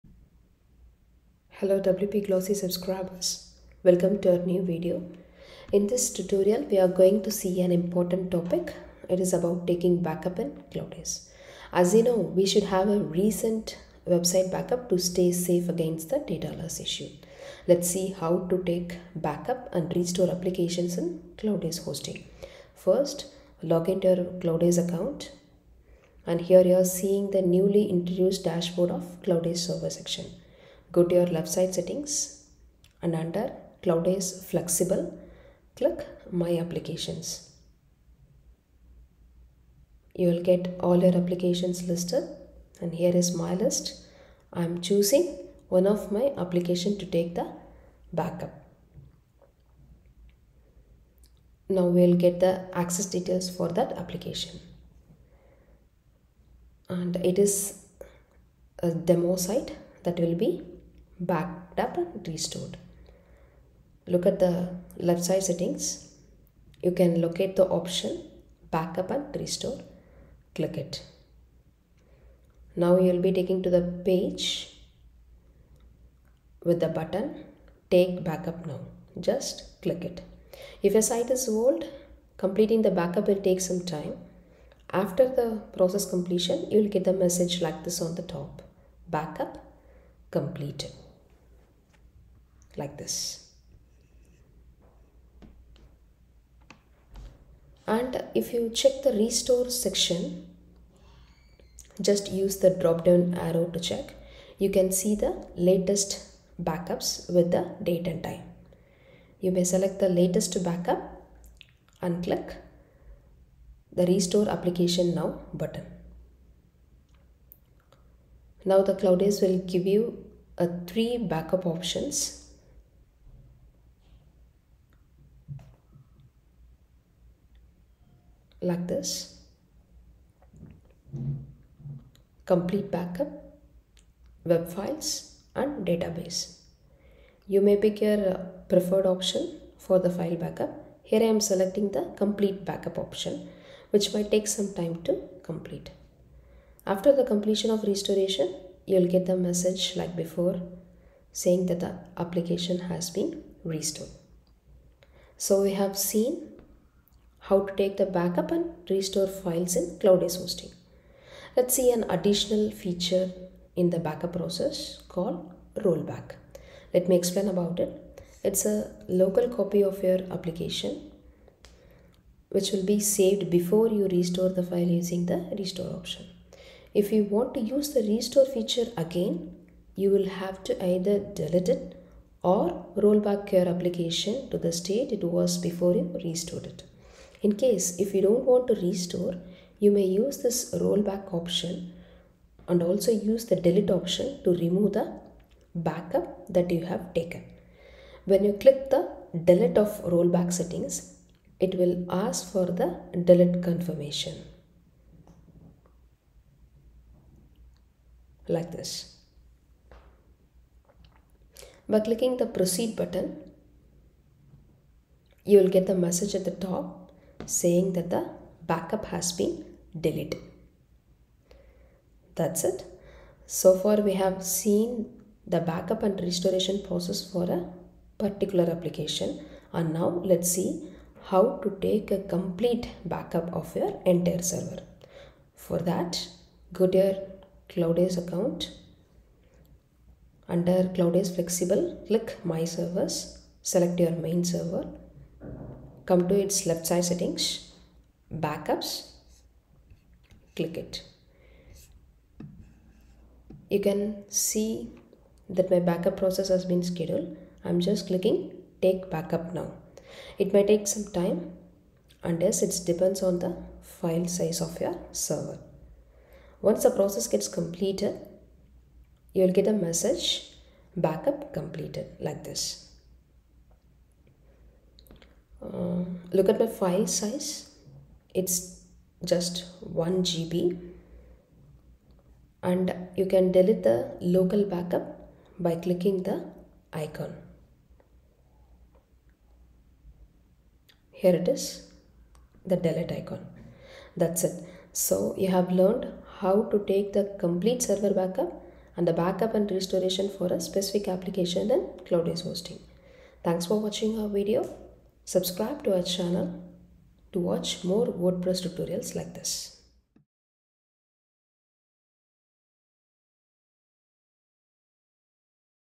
How To Take Backup And Restore Application In Cloudways?
All the Cloudways features are easy to use and yes, the backup too. Let's see how to take backup in Cloudways hosting!

Login to your Cloudways Account, Go to left side settings, Under Cloudways Flexible, Click My Applications and choose any of your applications to take backup.

It will take you to the Application settings section; click the Backup and Restore application option.

Now Click - Take Backup Now button. That's it! You will get the message - Backup Completed.

On the same page, you can see the settings for the file restoration. Choose the latest backup and click - Restore Application Now.

You will get three backup options; choose your preferred one.

You may use the "Rollback" feature to delete the backup.

And if you want to back up your entire server instead of a specific application, Go to Cloudways Flexible - My Servers. Go to its left-side settings, click Backups - Take Backup Now.